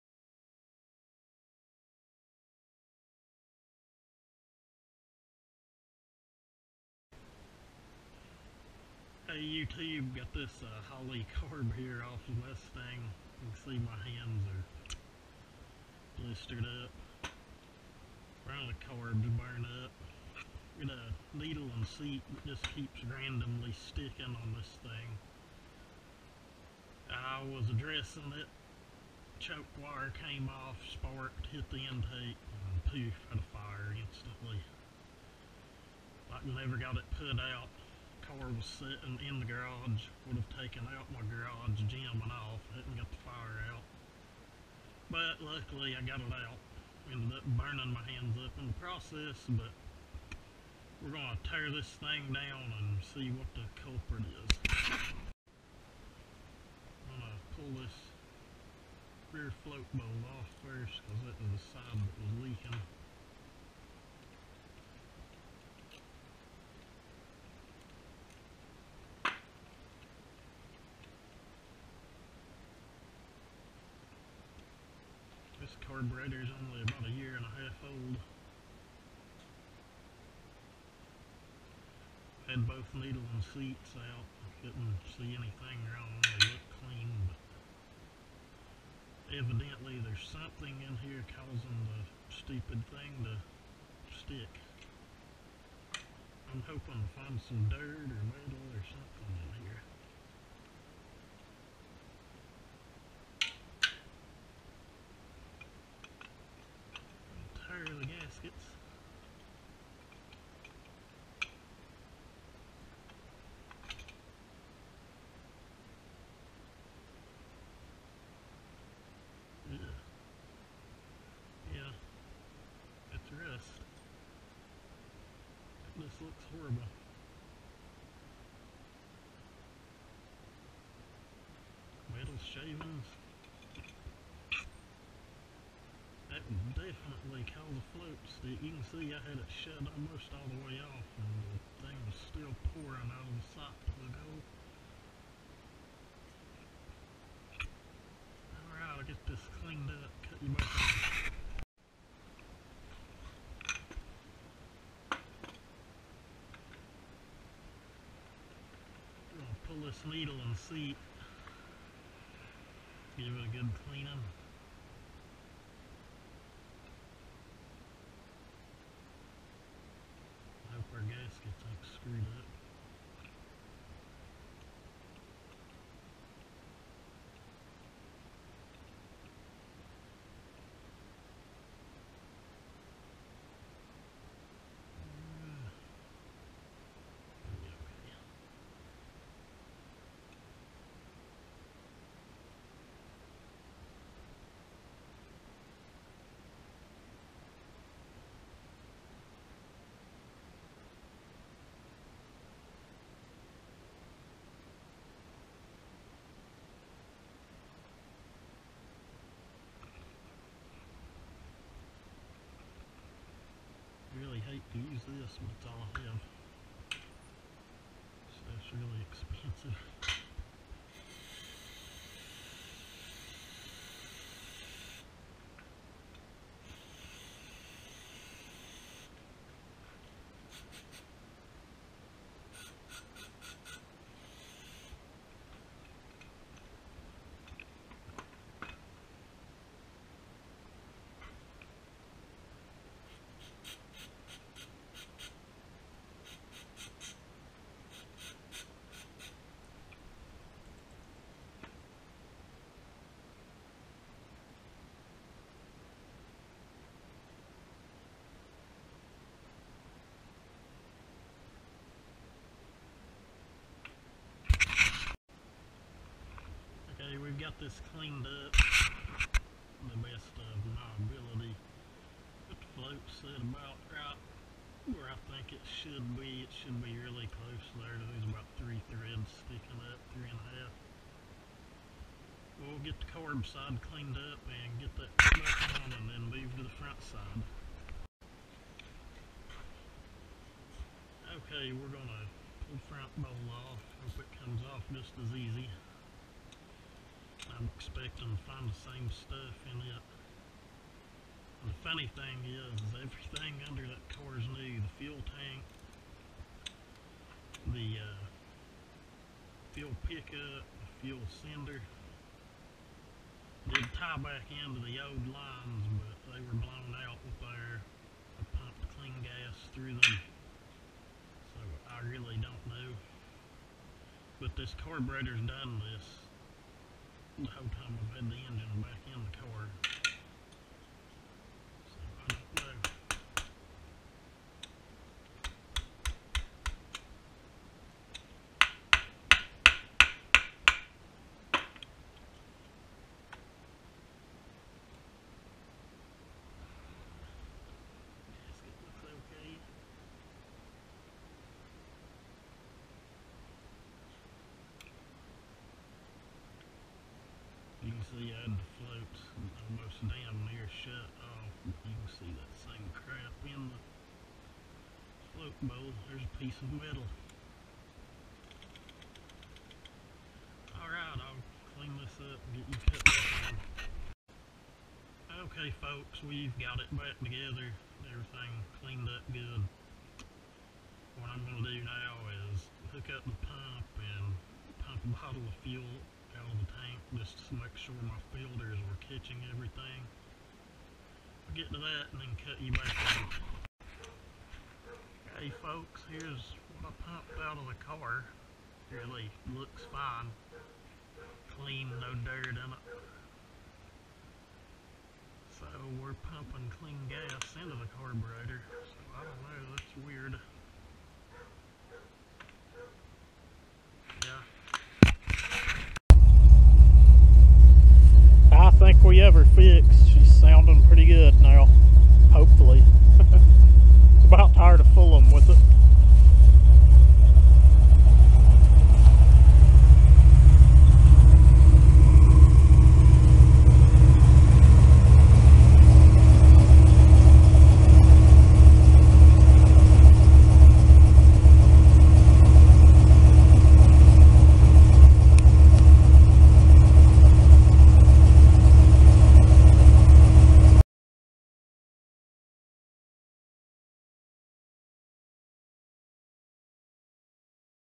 Holley Carberator Flooding Sticking needle and seat
in this video I pull the float bowls off the aluminum 750cfm holley carberator on my 351w swapped sn95 mustang...found the culprit of a problem where the needle and seat would randomly stick...what looked like rust and im assuming metal flashing from the inside of the carb.